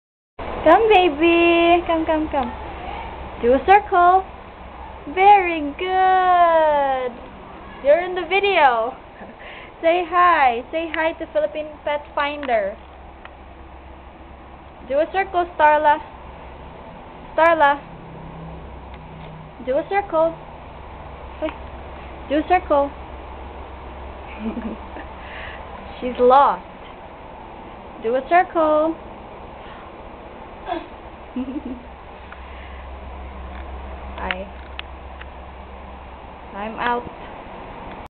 Starla and Skyla: Do A Circle Trick
My babies do the trick for cucumbers. Though Starla was lost but she did it while I was not paying attention. As you may have noticed, I got a red wire dog cage in there but I put a layer of cardboard, towel and fleece/cotton bedding on there and it serves as their bedroom since they like to have something over their head. Both girls are 10weeks old when this video was taken :) 12/05/09